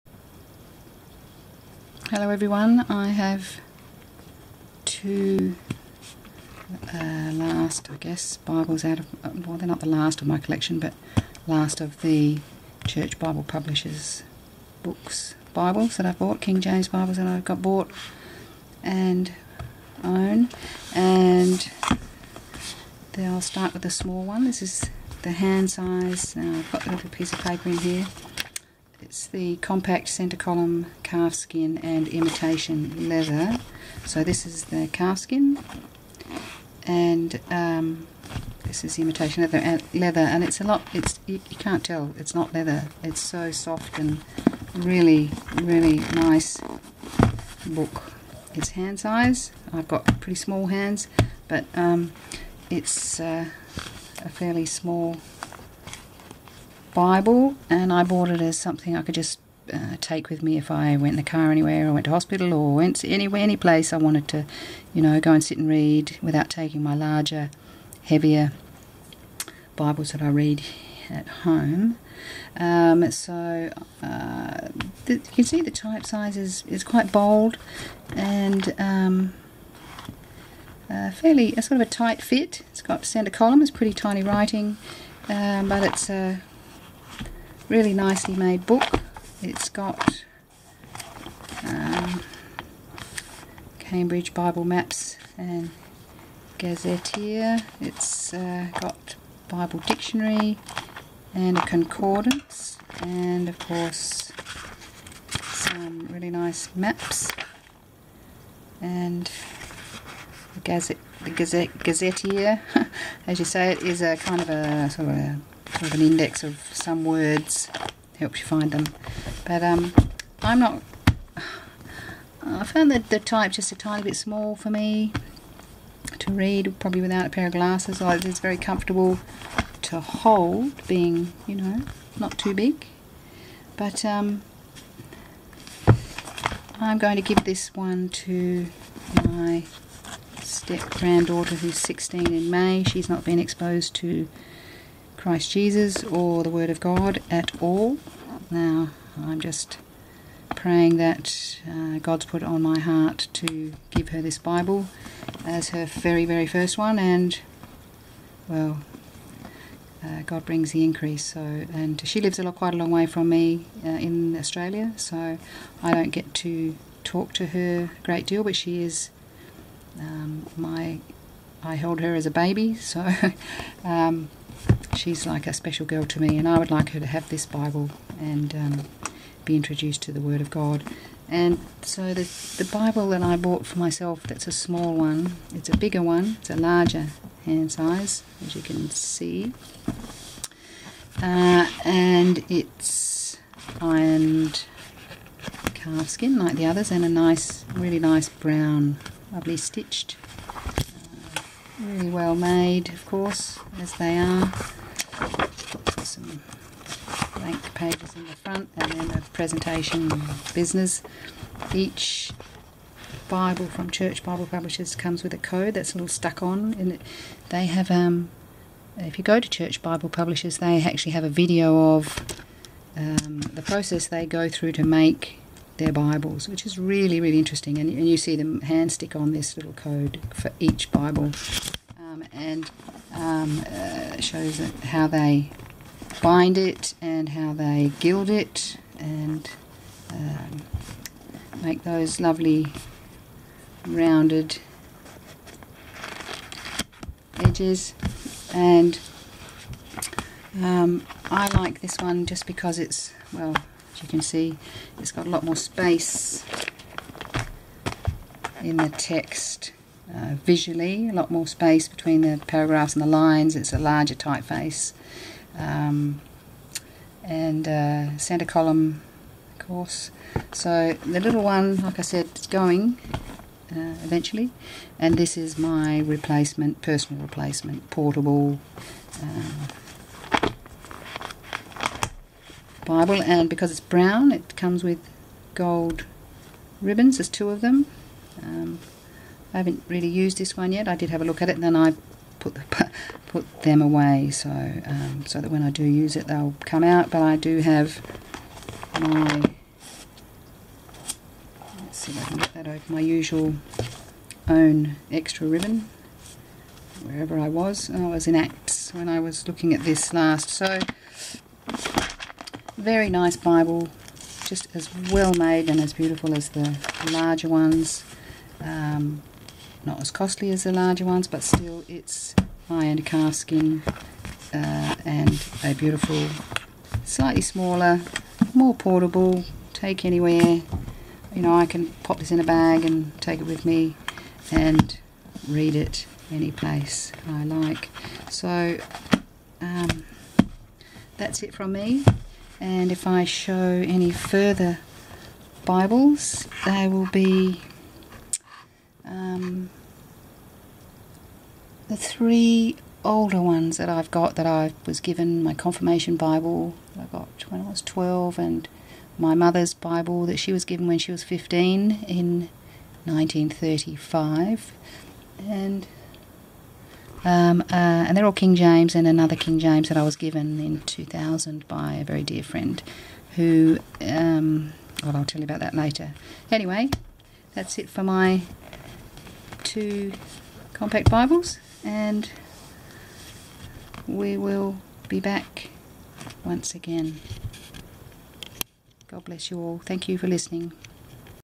My Two Hand Size King James Bibles
Both of these hand size King James bibles come from Church Bible Publishers in America